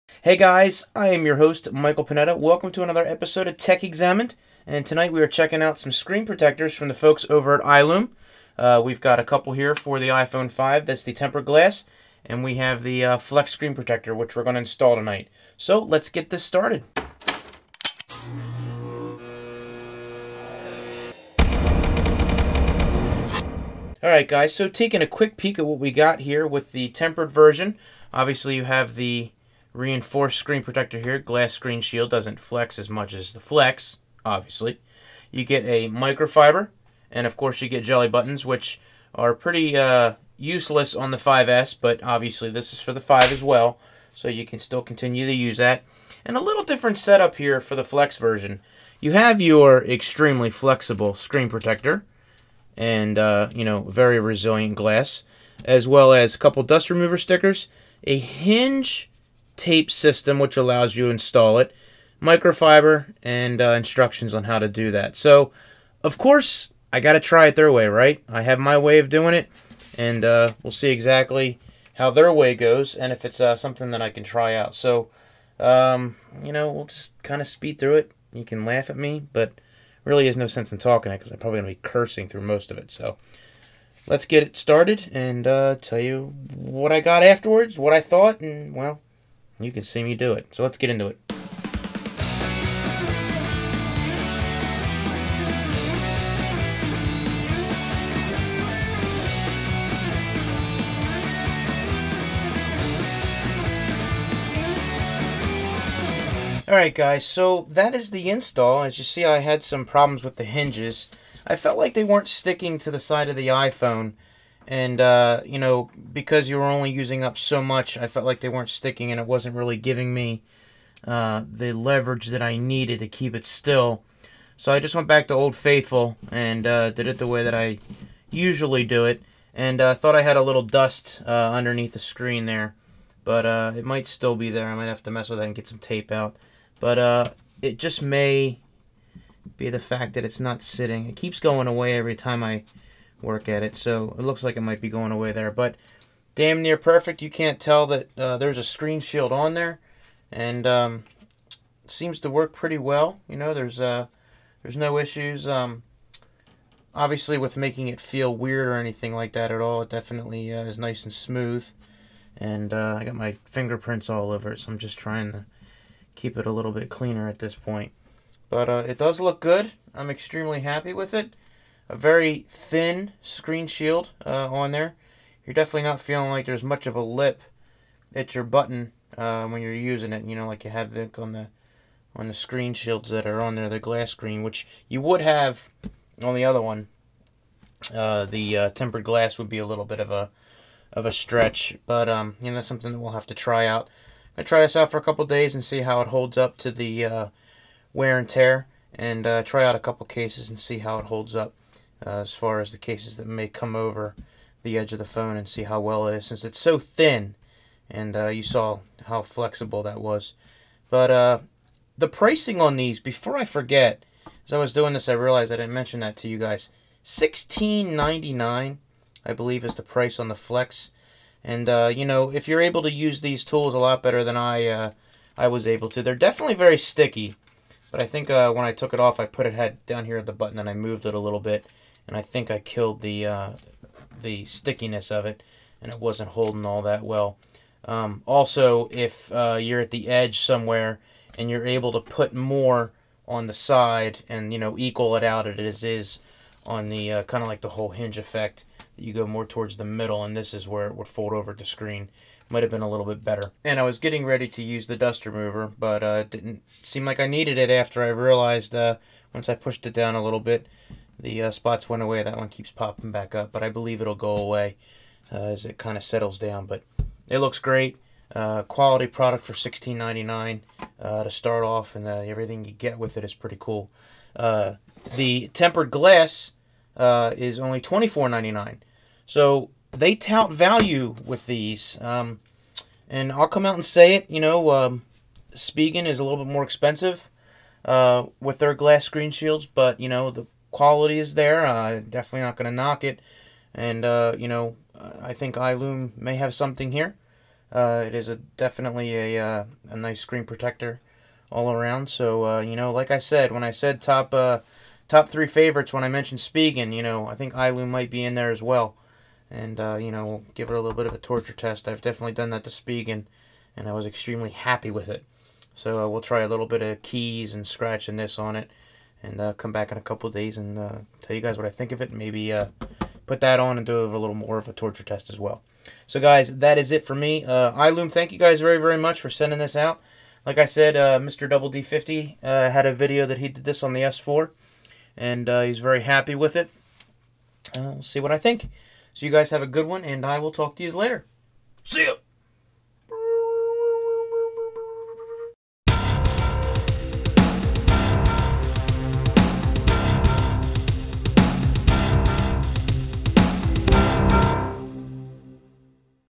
iLOOME SCREENSHIELDS- Flex & Tempered Glass iPhone
Big Thanks to iLoome for letting me try these out on the iPhone 5s
Wann pick these up here is out link to Amazon for each